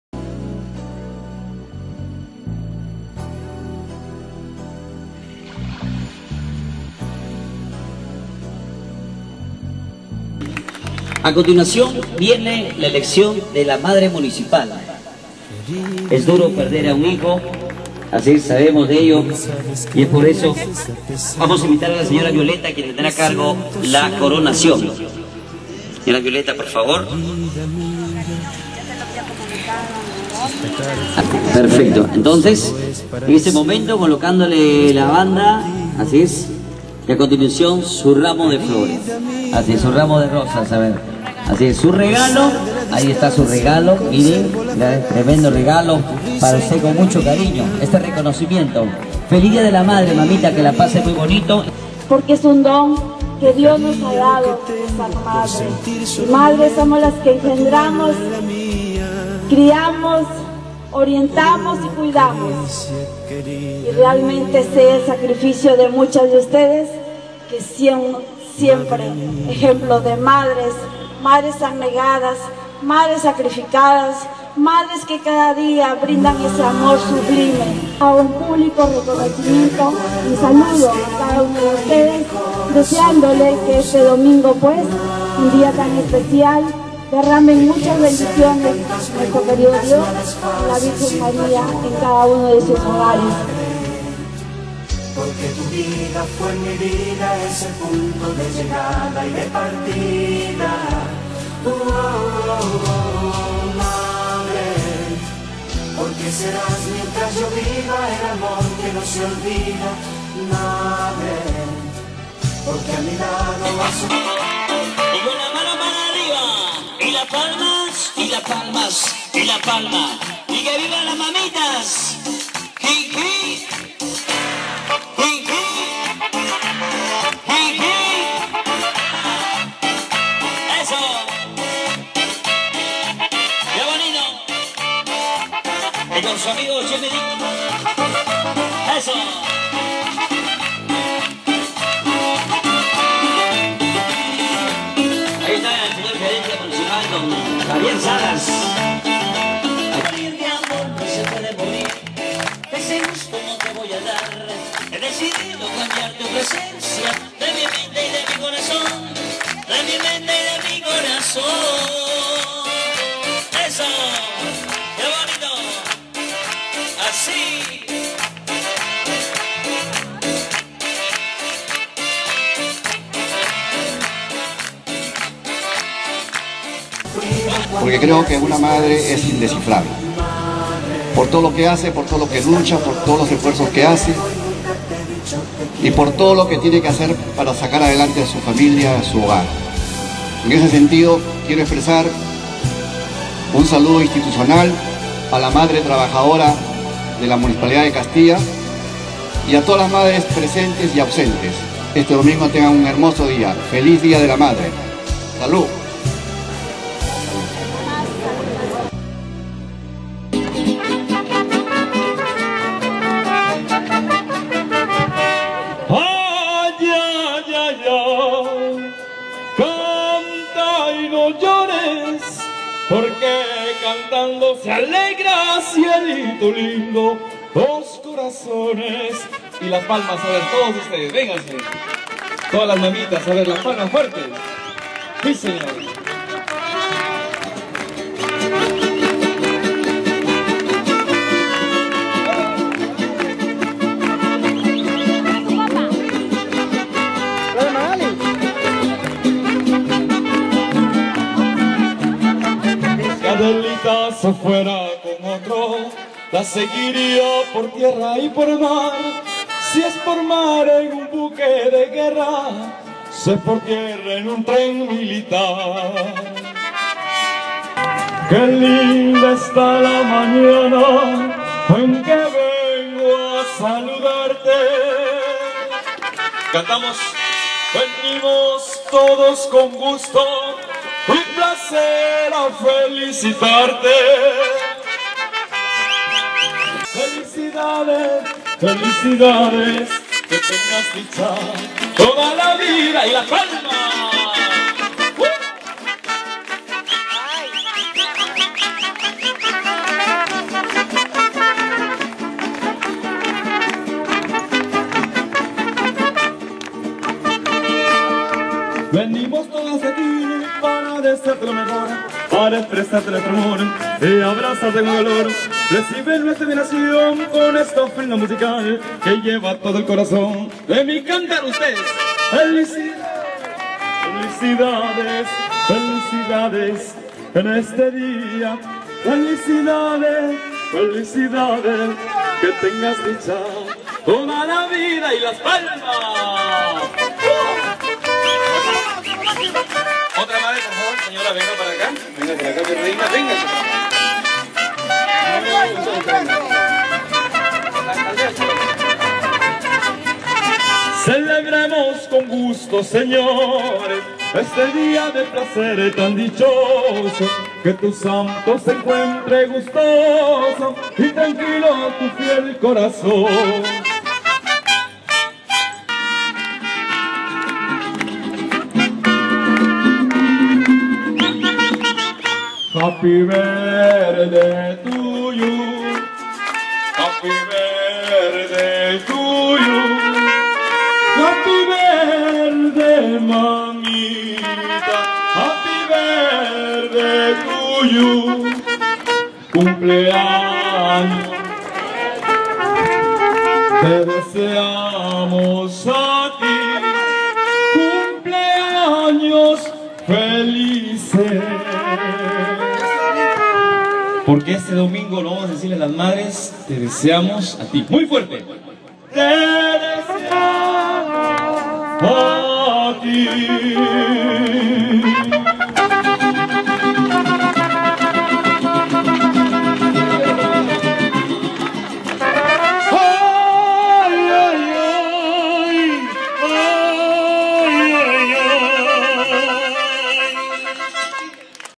Municipio de Castilla rinde Homenaje a la madre servidora municipal
Homenajeadas disfrutaron de una mañana de baile y mucha alegría.

CASTILLA. Más de 250 madres de familia que laboran en la Municipalidad Distrital de Castilla festejaron su día, en una ceremonia especial que organizó la comuna en la plazuela Luis Montero de esta ciudad.

Las homenajeadas disfrutaron de una mañana de baile, música y mucha alegría. Asimismo, recibieron el saludo institucional de la alcaldesa Violeta Ruesta de Herrera, quien en compañía de sus regidores hicieron entrega de canastas de víveres y regalos, además de una tarjeta alusiva a la fecha.

La trabajadora municipal Raquel Márquez Juárez, fue elegida como Madre Municipal 2014, por tal mérito se le entregó presentes y una banda distinguiéndola como representante de las madres municipales.